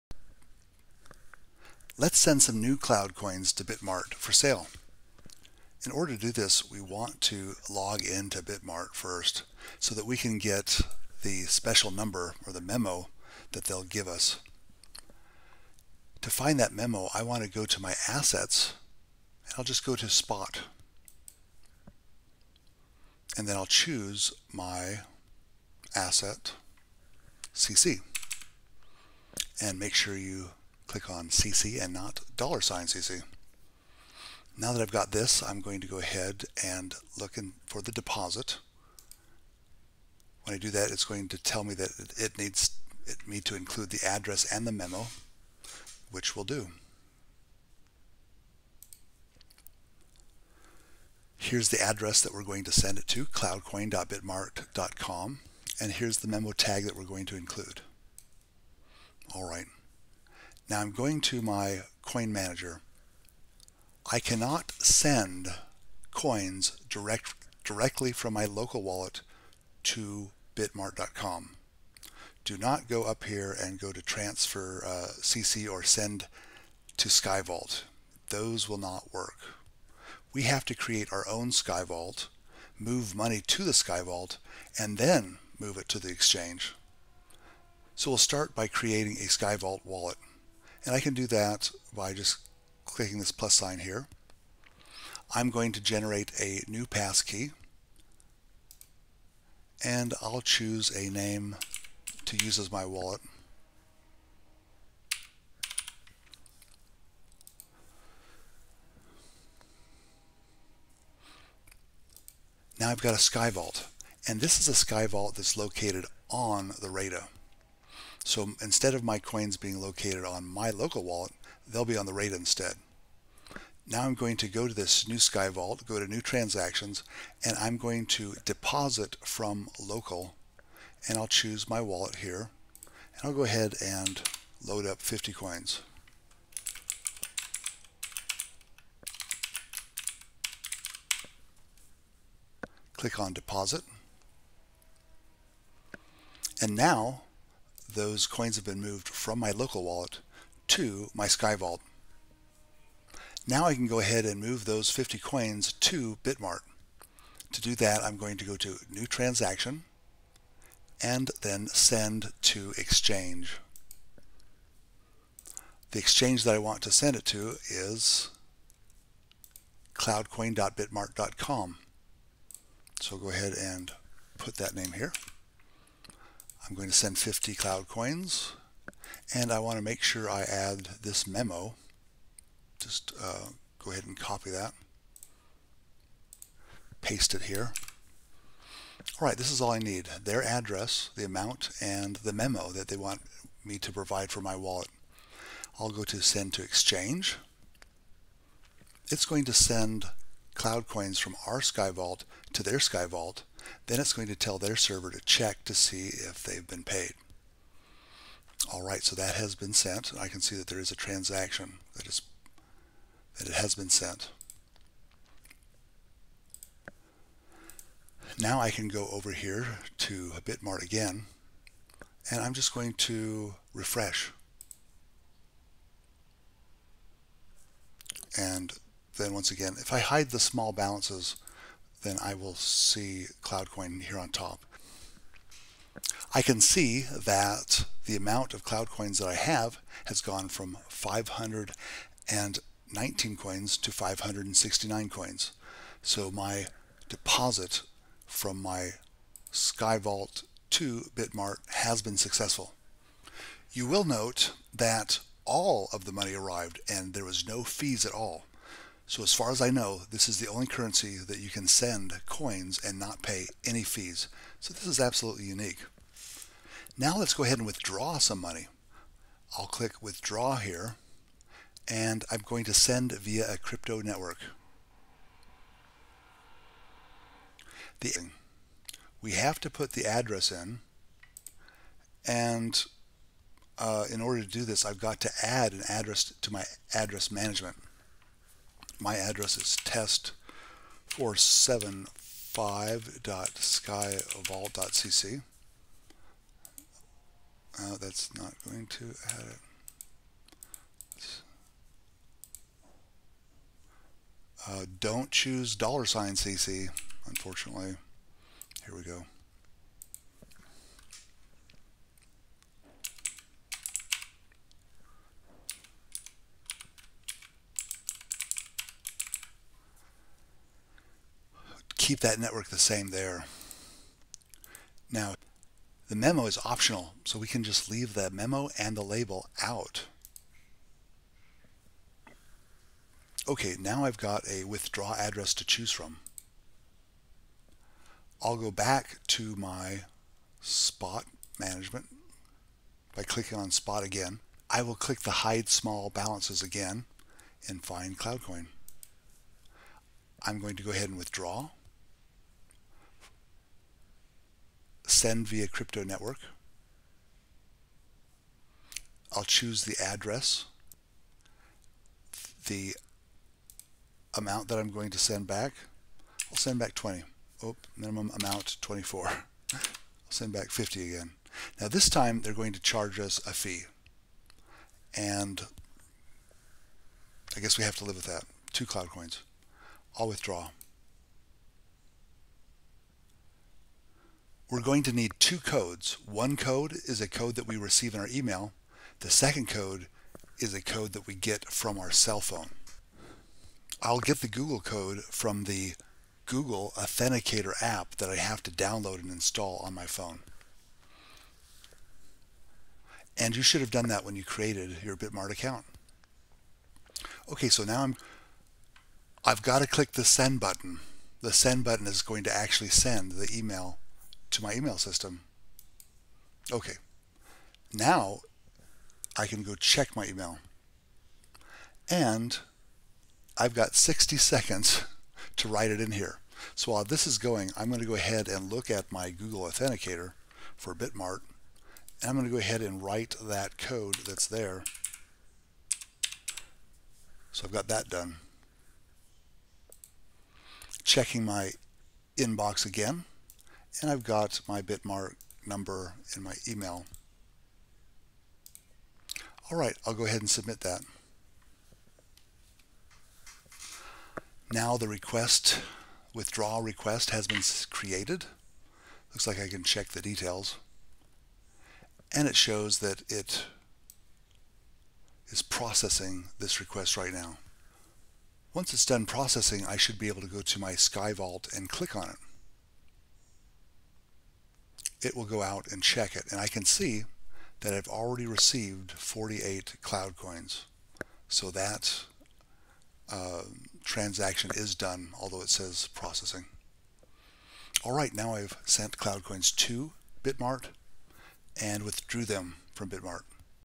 Deposit to and withdraw from Bitmart.
This video demonstrates how to use the Coin Manager to deposit CloudCoin 2.0 to the Bitmart.com exchange. And, how to withdraw coins from the Bitmart.com exchange and into your Coin Manager.